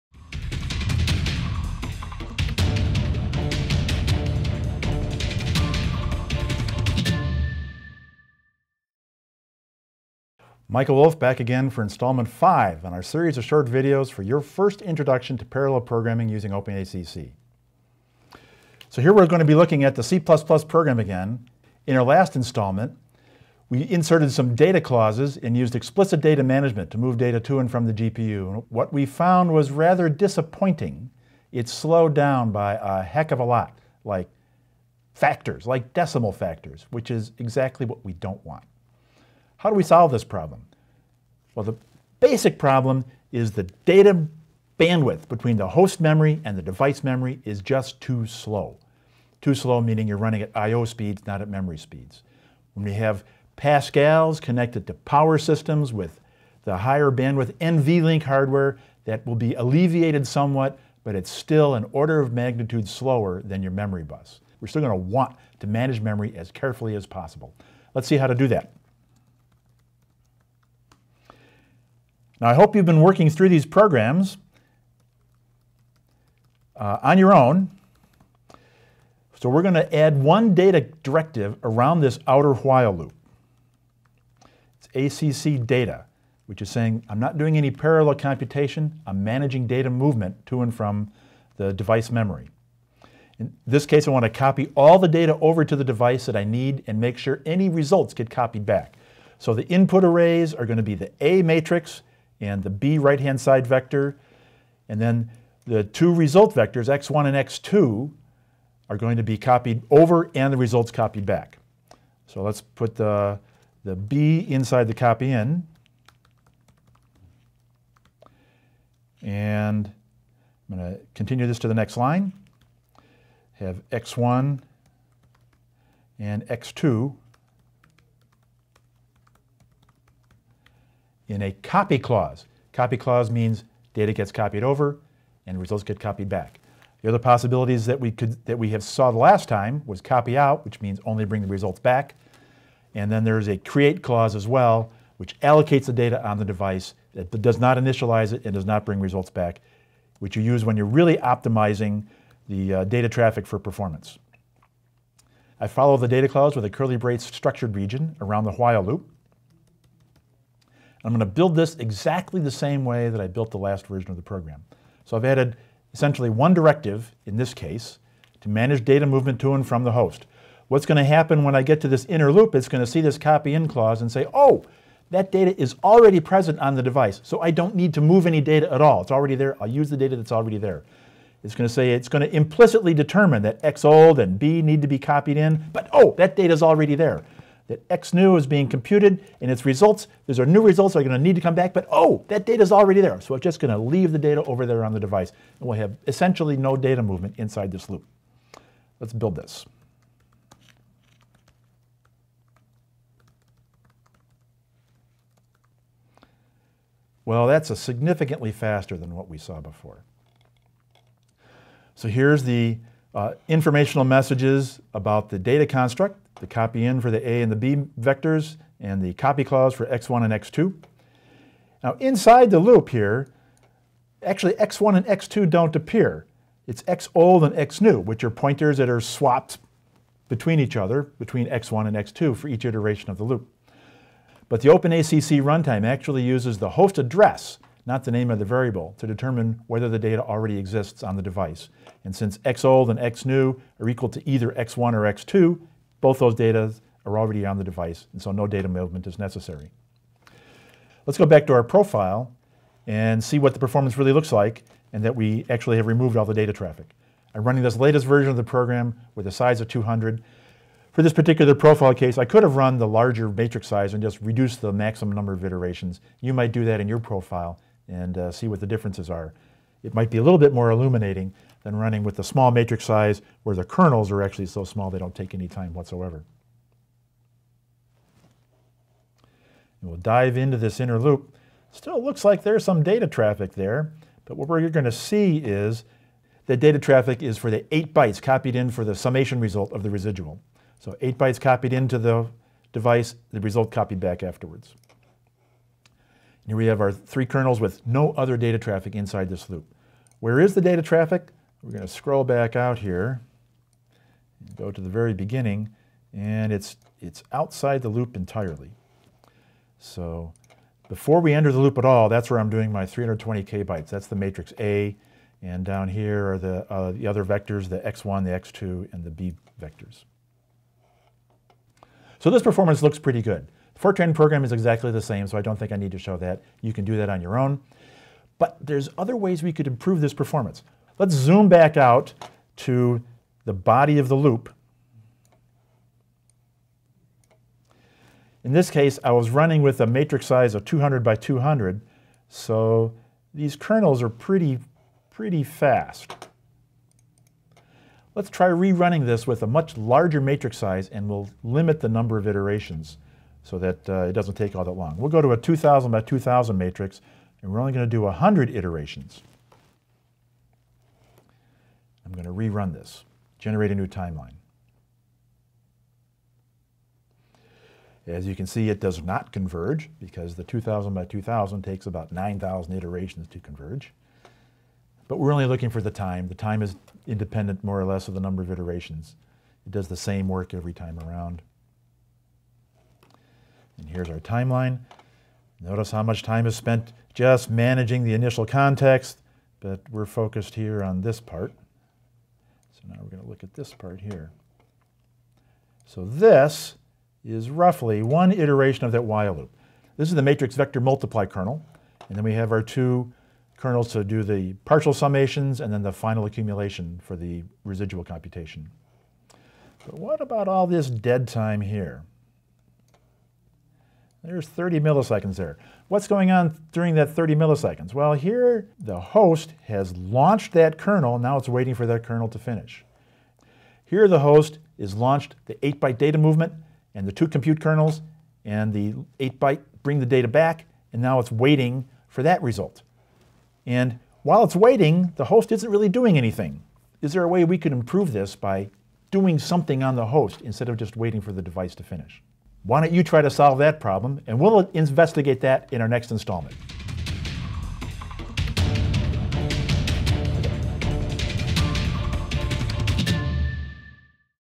Introduction to Parallel Programming with OpenACC - Part 5
The fifth in a series of short videos to introduce you to parallel programming with OpenACC and the PGI compilers, using C++ or Fortran.  You will learn by example how to build a simple example program, how to add OpenACC directives, and to rebuild the program for parallel execution on a multicore system.  To get the most out of this video, you should download the example programs and follow along on your workstation.